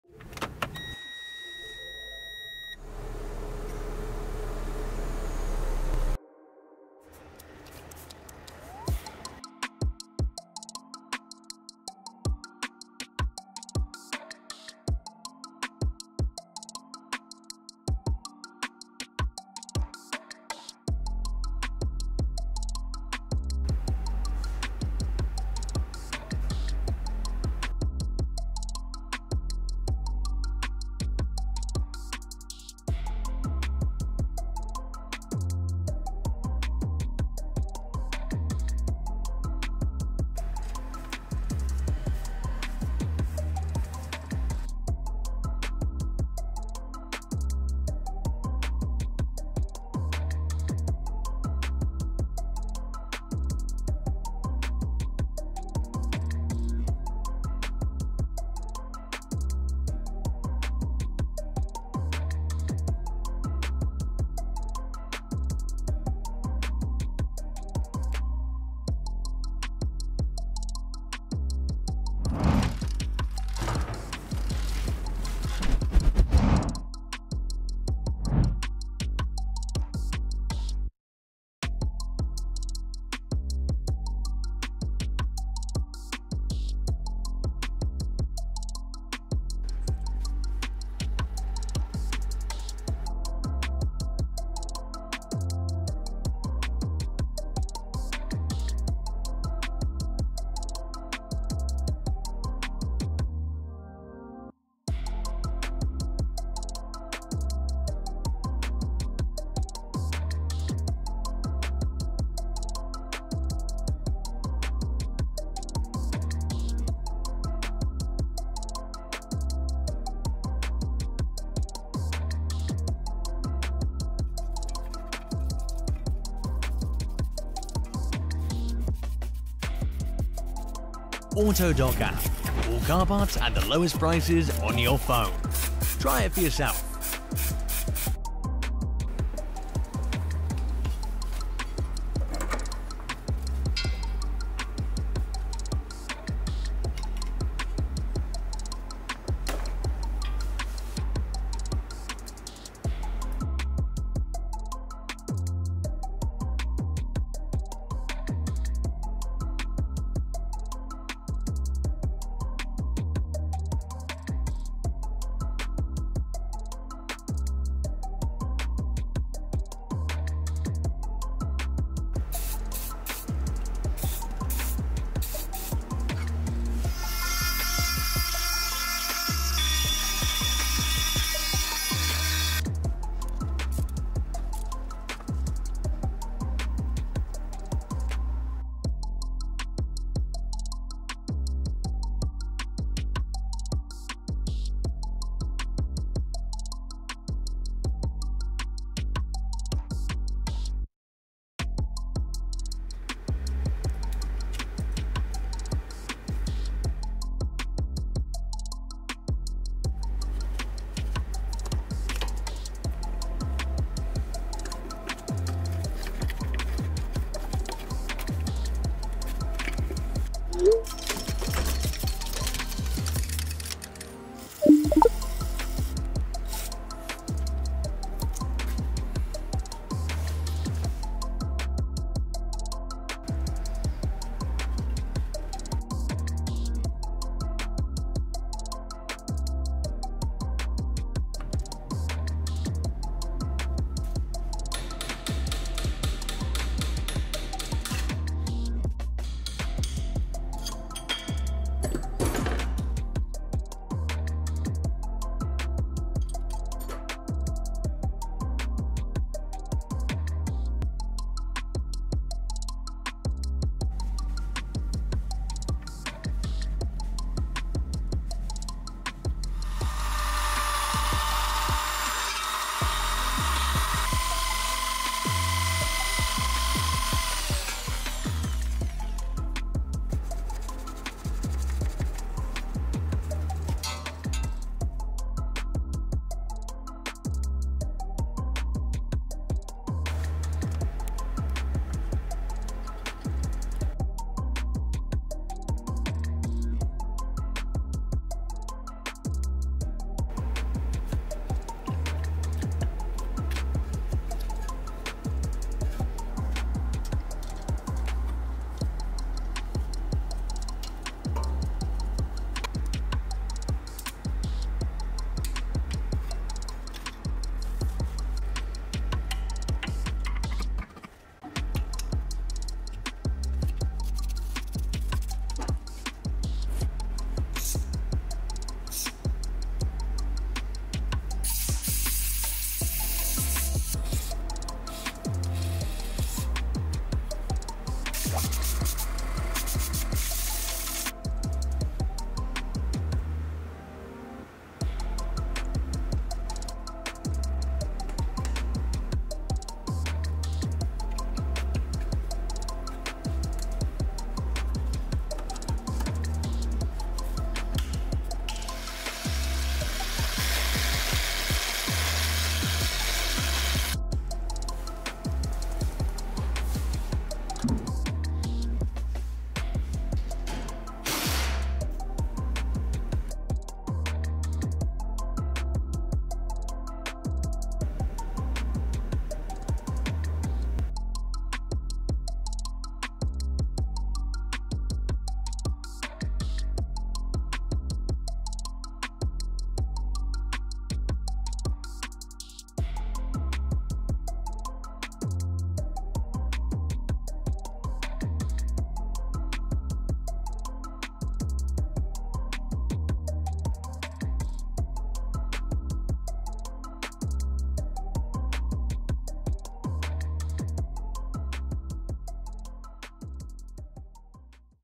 How to change a wheel speed sensor [AUTODOC TUTORIAL]
⚡ All video and PDF tutorials on parts: download and watch at AUTODOC CLUB

00:00 — ABS sensor / wheel speed sensor: step-by-step changement tutorial
01:37 — Things you may need
02:35 — Remove the ABS sensor connector
03:03 — Unscrew the ABS sensor fastener
03:14 — Remove the sensor from its mounting seat
04:54 — Install the new sensor
05:41 — Attach the connector and secure the sensor wiring harness